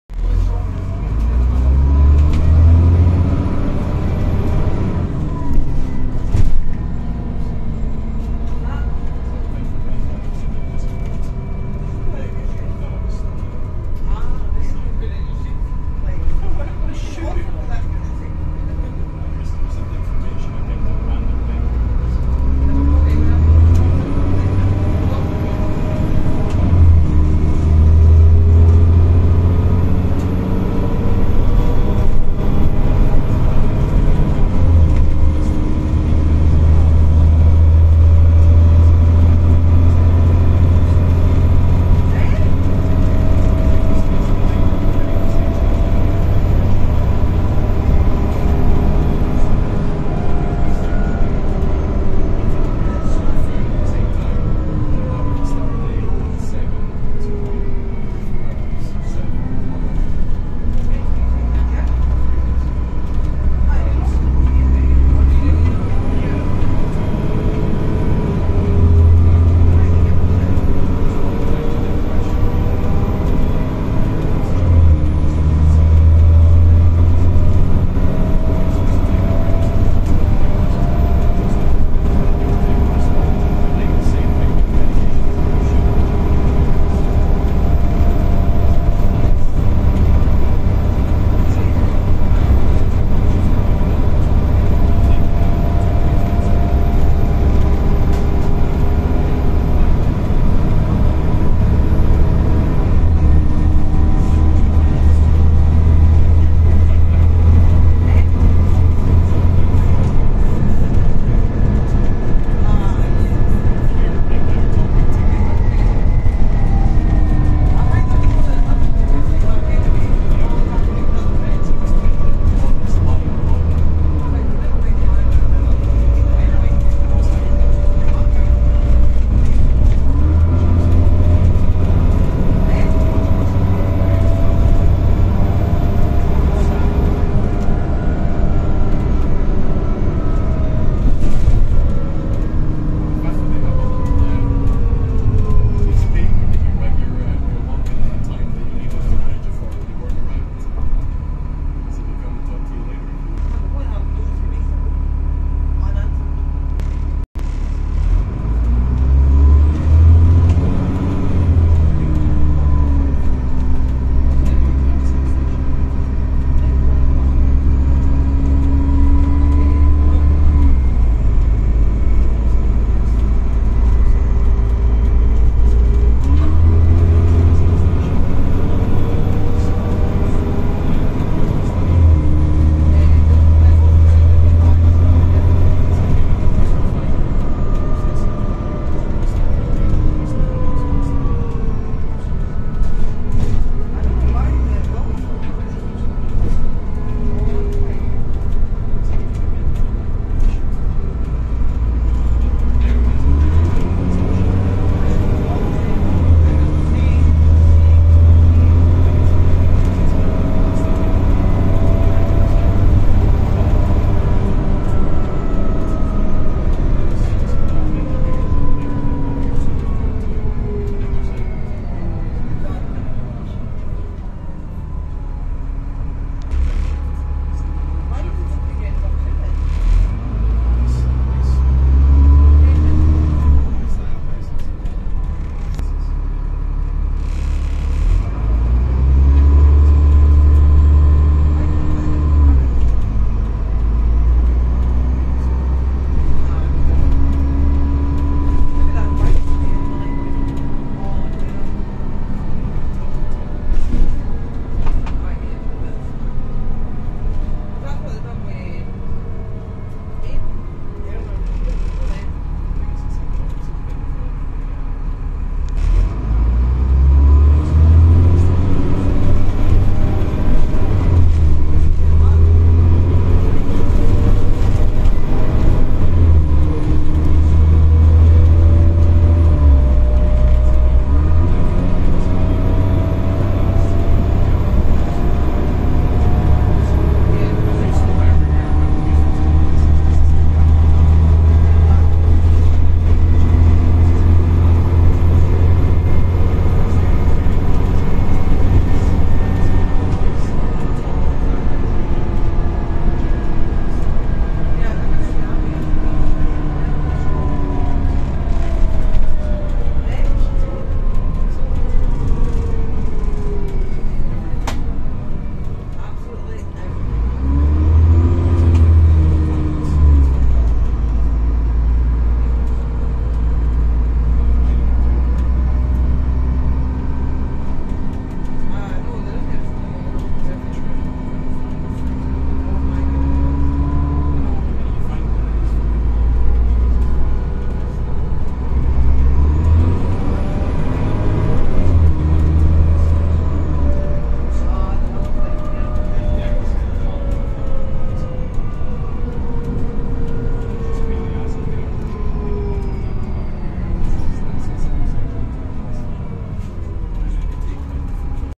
*Kickdowns!!* Stagecoach fife 18071 ALX400 Dennis trident (WA04 CSU)
In this video we get 18071 on a kickdown packed run on the AZ1 from Dunfermline - hallbeath park and ride

Kickdowns: 0:22, 1:03, 2:08, 2:55, 3:22, 4:25, 4:39, 5:38